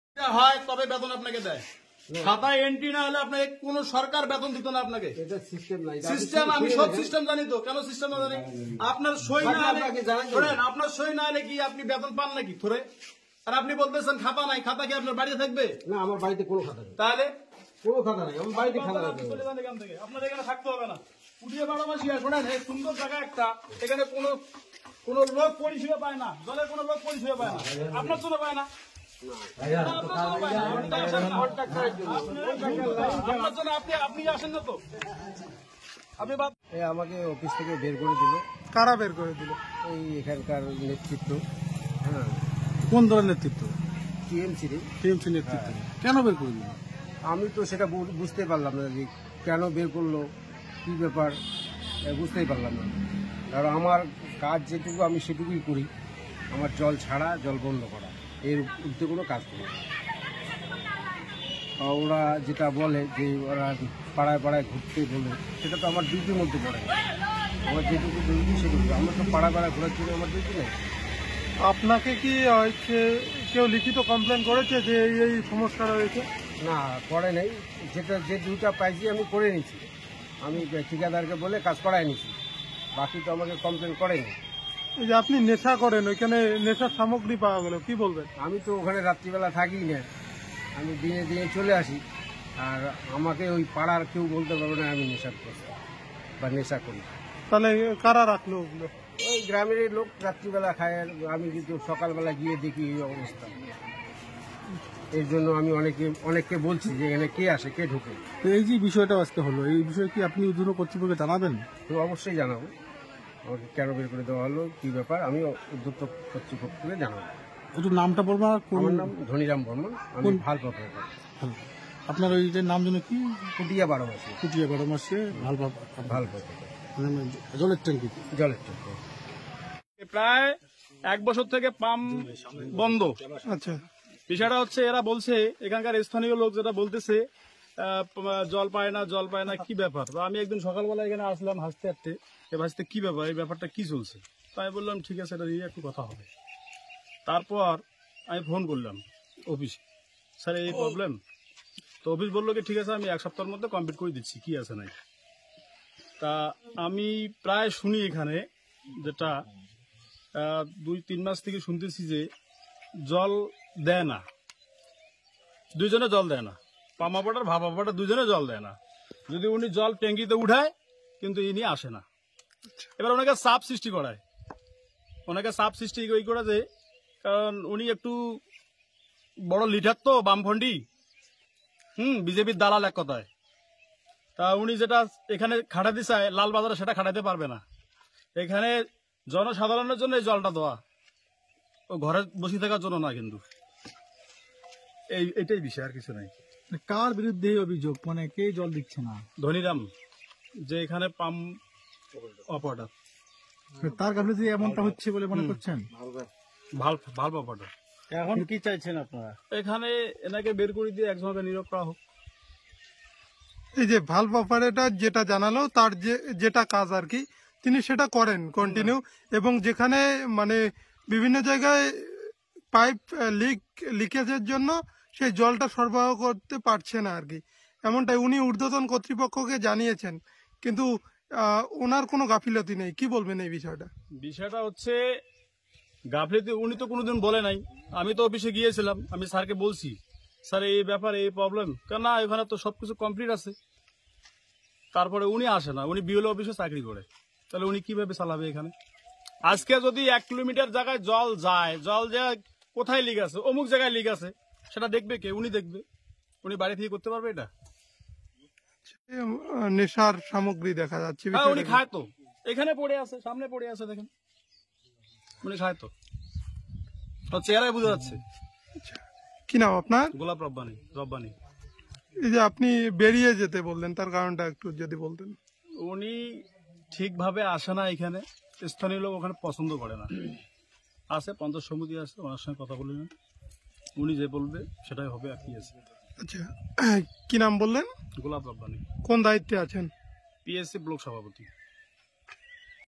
ভালব অপারেটরের গাফিলতিতে জল পাচ্ছে না এলাকাবাসী। যদিও অভিযোগ অস্বীকার ভালব অপারেটর ধনিরাম বর্মনের।
ভালব অপারেটরের গাফিলতিতে জল পাচ্ছে না এলাকাবাসী। এই অভিযোগ তুলে বিক্ষোভ, যদিও অভিযোগ অস্বীকার ভালব অপারেটর ধনিরাম বর্মনের।